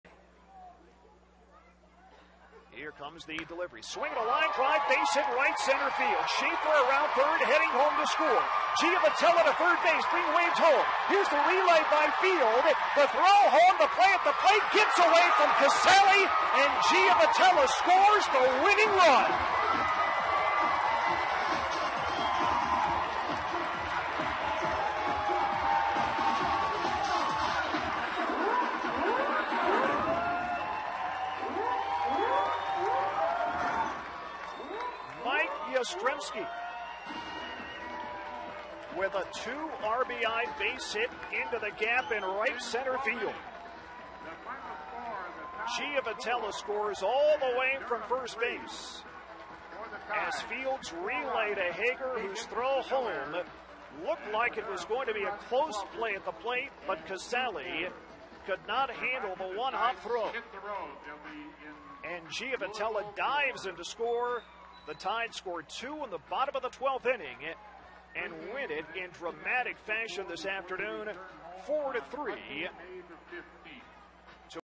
Mike Yastrzemski wins it for the Tides
5/7/17: Mike Yastrzemski delivers in the clutch to give Norfolk a 4-3 win over Durham